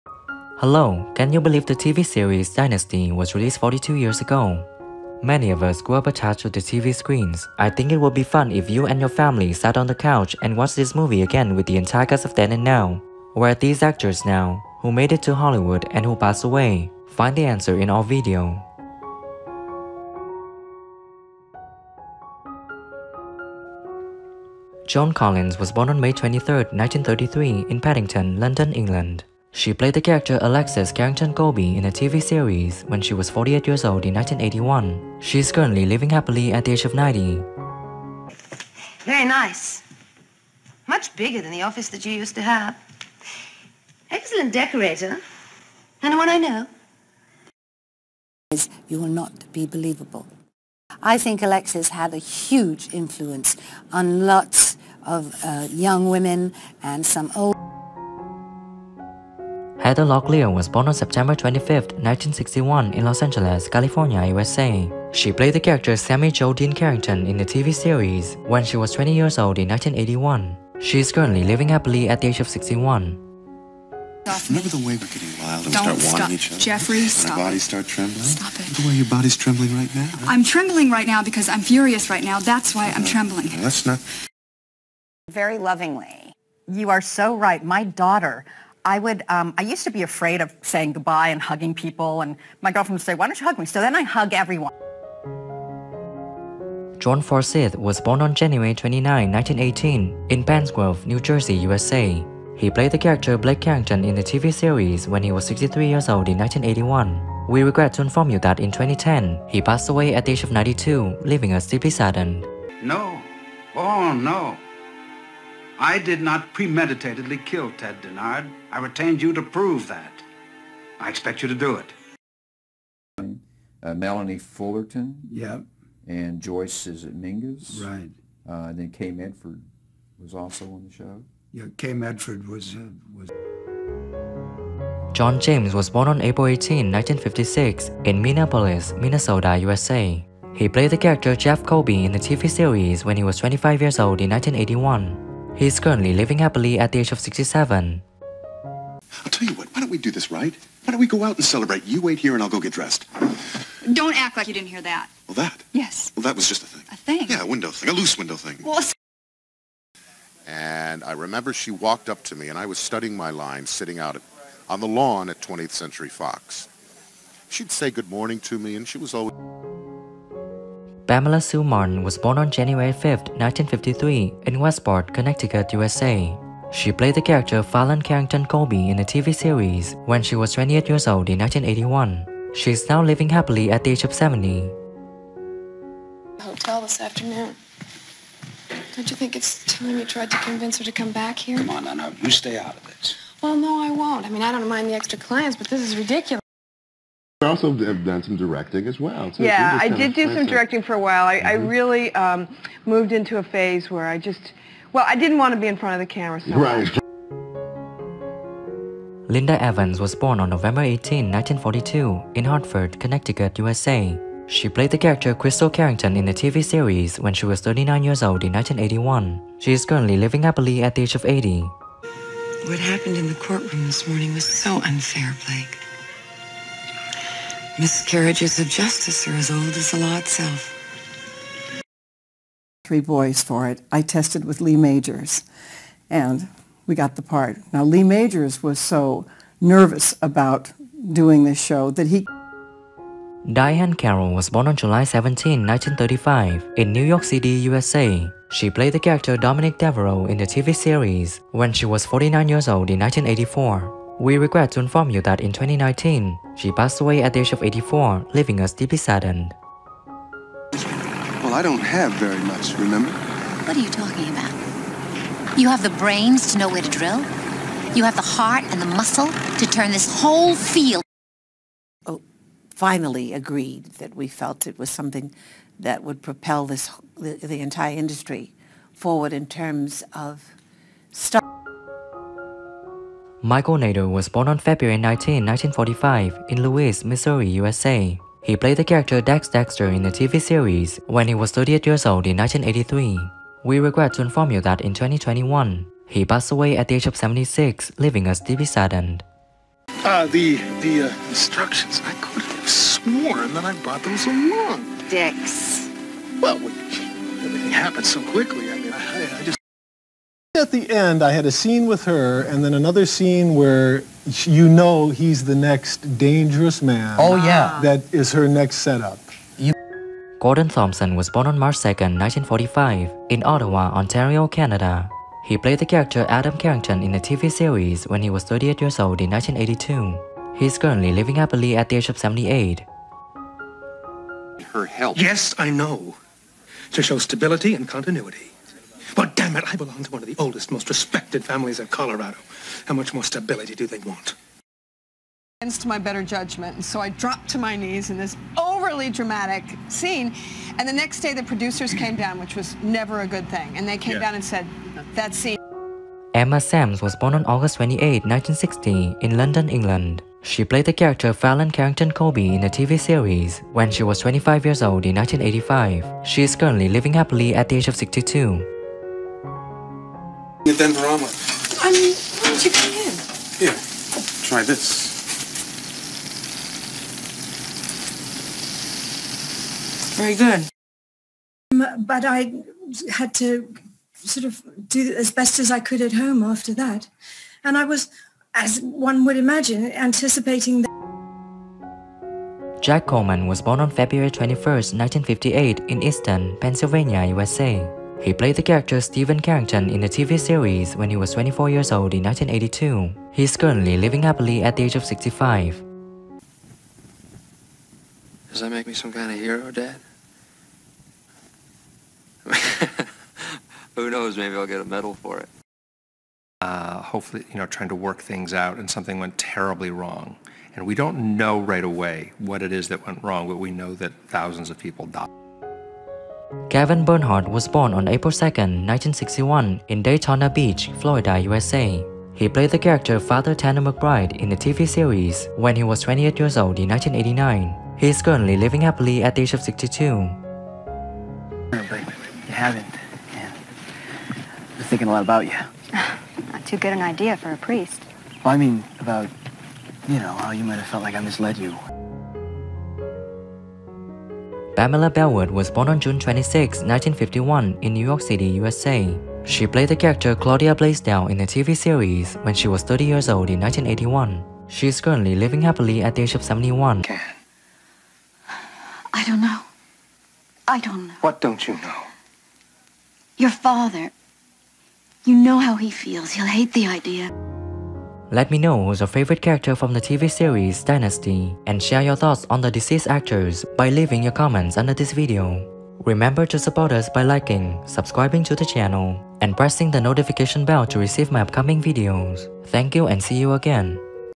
Dynasty (1981–1989) Cast: Then and Now 2023 Who Passed Away After 42 Years?
Dynasty is an American prime time television soap opera that aired on ABC from January 12, 1981, to May 11, 1989. The series, created by Richard and Esther Shapiro and produced by Aaron Spelling, revolves around the Carringtons, a wealthy family residing in Denver, Colorado. Dynasty stars John Forsythe as oil magnate Blake Carrington, Linda Evans as his new wife Krystle, and later Joan Collins as his former wife Alexis.